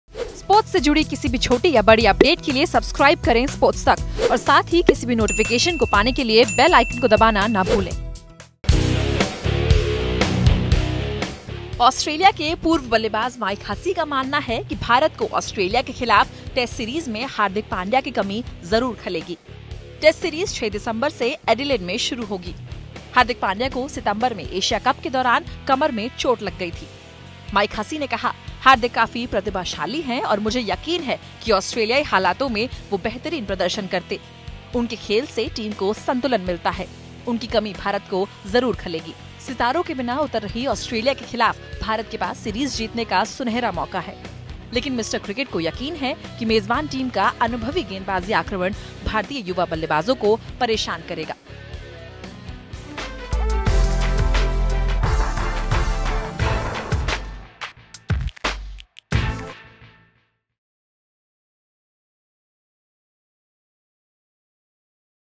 Hardik Pandya's game was suited for Australian conditions, his absence will hurt India: Hussey
Mike Hussey feels that all-rounder Hardik Pandya's absence will "hurt" the visitors in the upcoming Test series against Australia.
The series starts on December 6 in Adelaide.